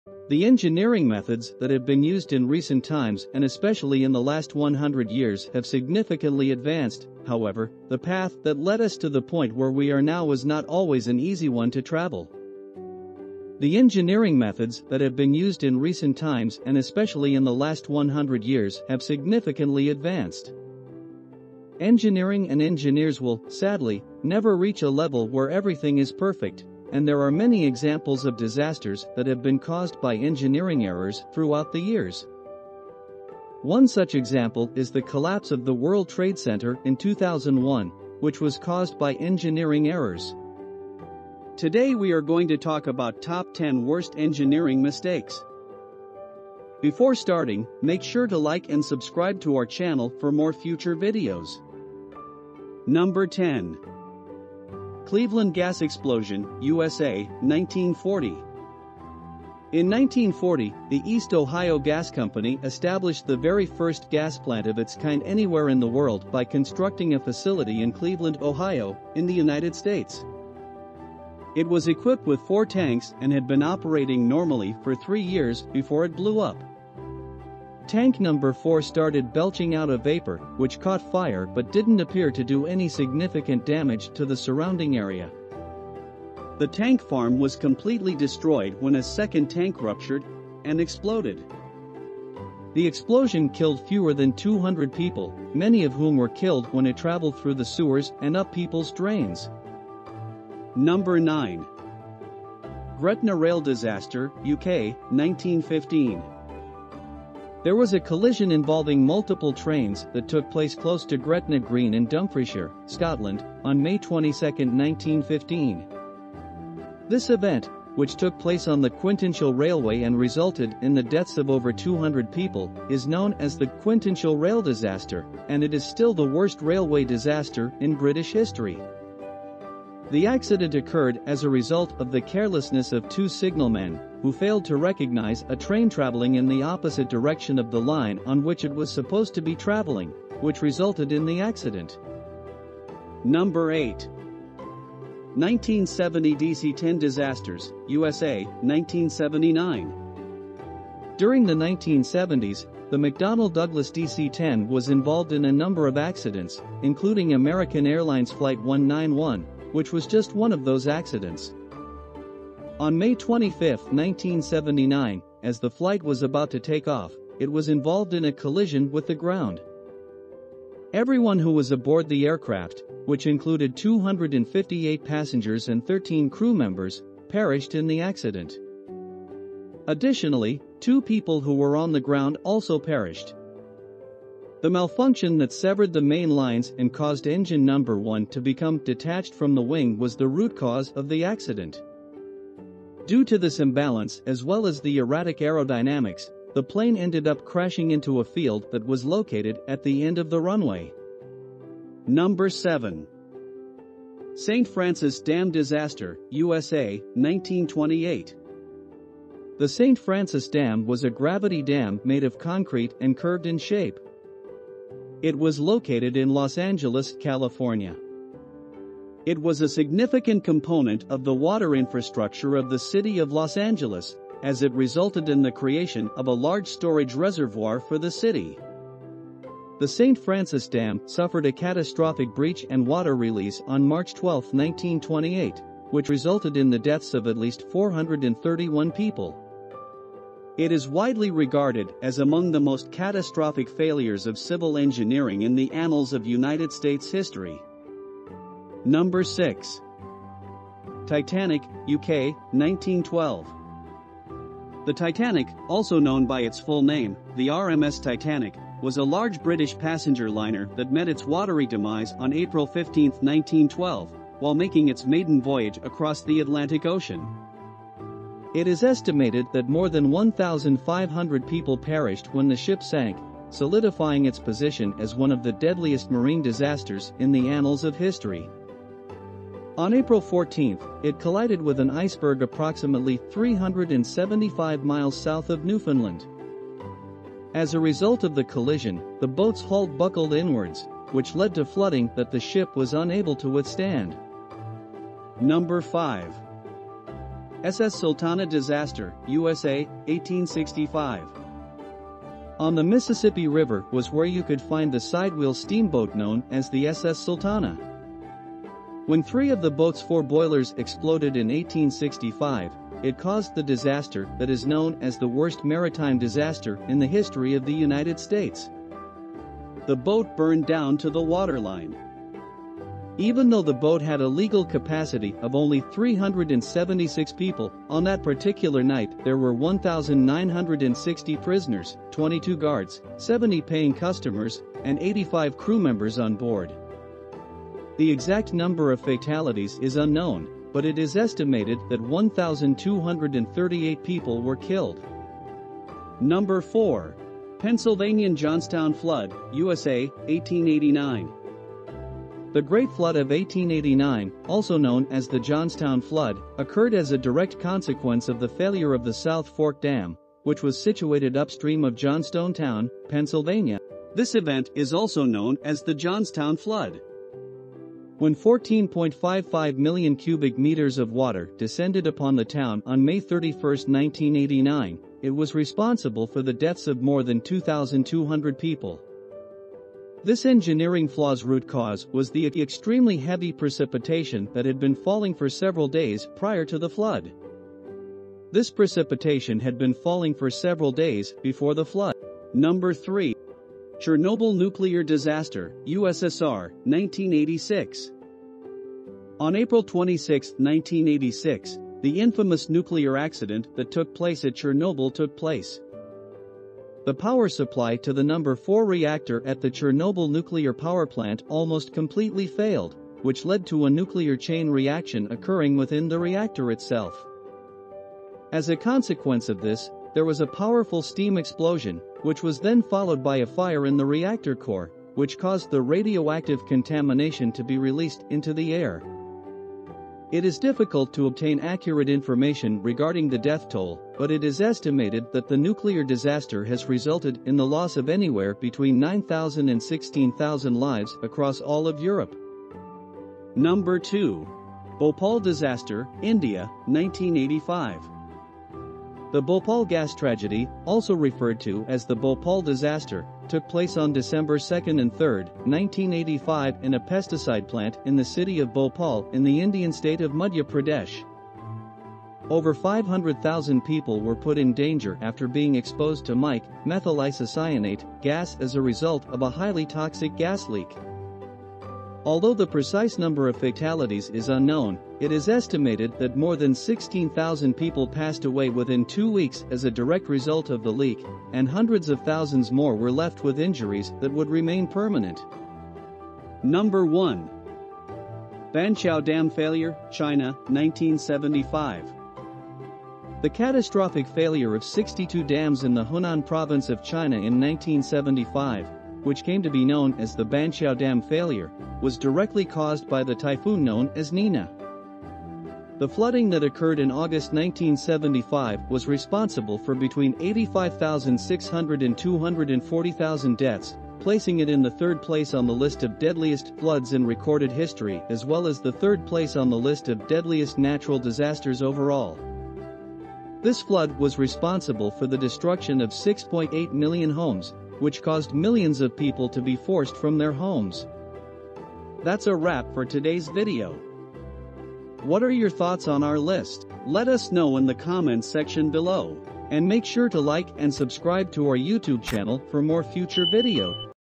The 10 Most Shocking Engineering Disasters – You'll Never Believe What Happened!
Top 10 worst engineering mistakes that ended very badly. What do you think happened?

Do you think any of these 10 mistakes could have been avoided?

NOTE: This Video Was Created and Edited By "Tops & Life" So It Belongs Only to This Channel.